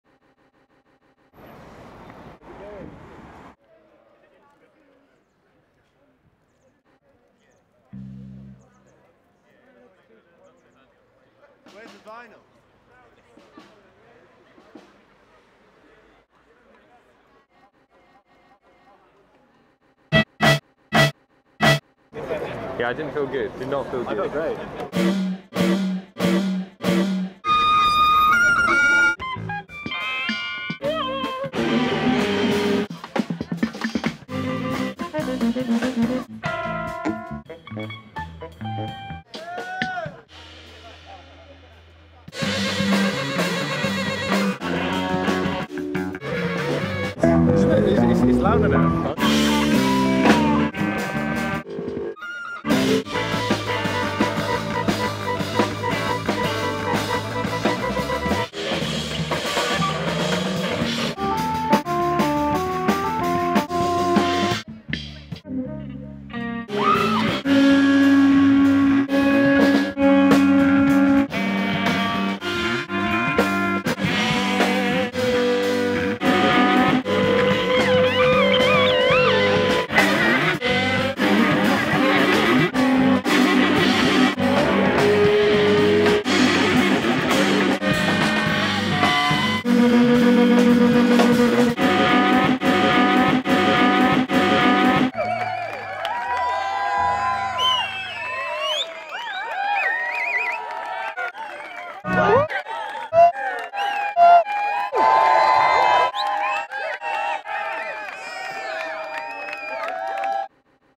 The Evil Usses @ Turbo Island
The Evil Usses album launch @ Turbo Island, Stokes Croft, Bristol. 25/09/15.